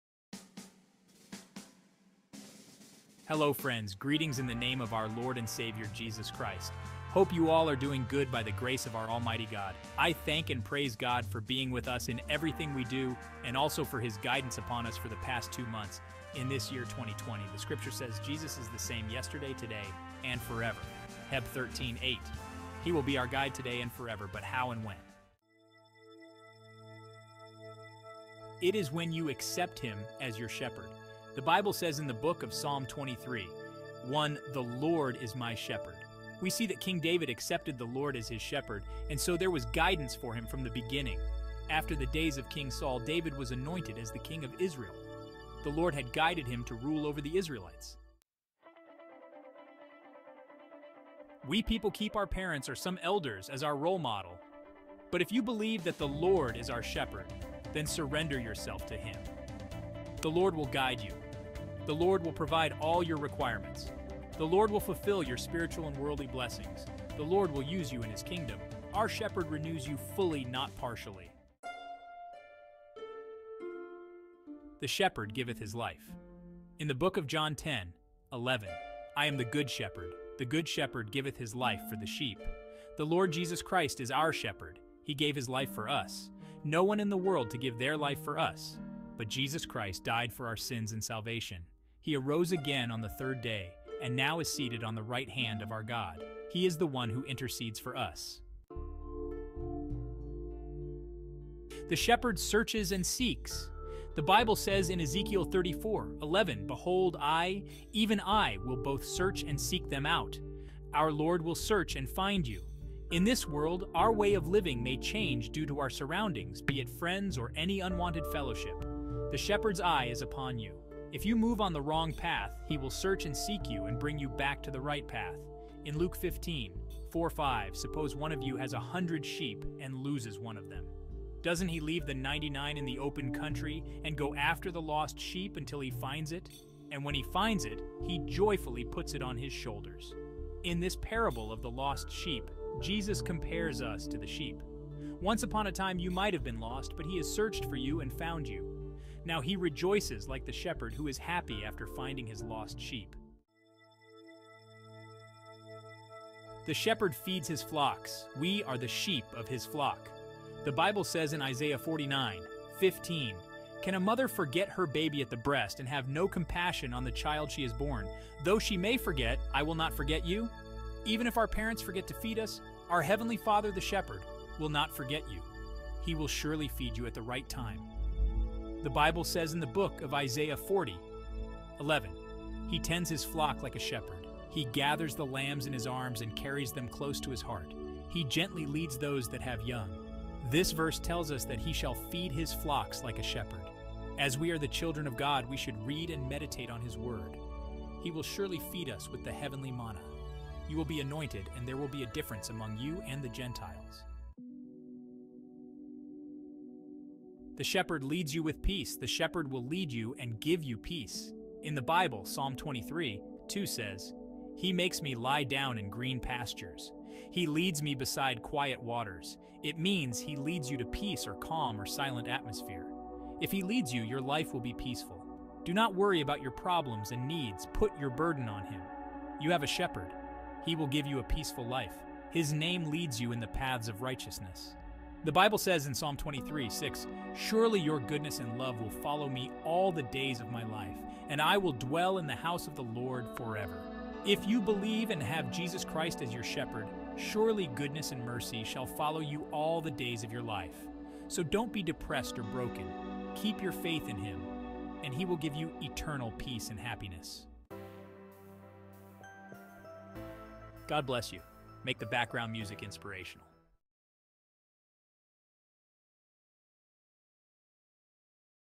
Finding Peace with Your Shepherd | Inspirational Message | J T Festus Jeyamon
Discover the Power of God's Guidance in Your Life

In this thought-provoking video, we'll explore the profound importance of allowing God, our Heavenly Shepherd, to guide us through the ups and downs of our daily lives. Drawing insights from the life of King David and the teachings of Jesus Christ, we'll uncover the transformative impact of surrendering to the Lord's divine direction.

Through engaging stories and biblical principles, you'll learn how to cultivate a deeper relationship with the Good Shepherd, who promises to provide unwavering support, protection, and purpose. Discover the peace, blessings, and fulfillment that come from trusting in the Shepherd's leading, even in the face of life's challenges.

Whether you're seeking clarity, strength, or a renewed sense of purpose, this video offers a timely reminder of God's steadfast commitment to guide and nurture His flock. Unlock the treasures of His word and experience the joy of walking in the Shepherd's footsteps.

We hope this video encourages you to deepen your trust in the Lord and embrace the transformative power of His guidance.

OUTLINE:

00:00:00 DO YOU HAVE A SHEPHERD?
00:00:25 Accepting the Shepherd
00:00:48 The Shepherd's Guidance
00:01:11 The Shepherd's Sacrifice
00:01:38 The Shepherd Searches and Seeks
00:02:33 The Shepherd Feeds His Flocks
00:03:24 The Shepherd Leads You With Peace
00:04:17 Conclusion and Blessings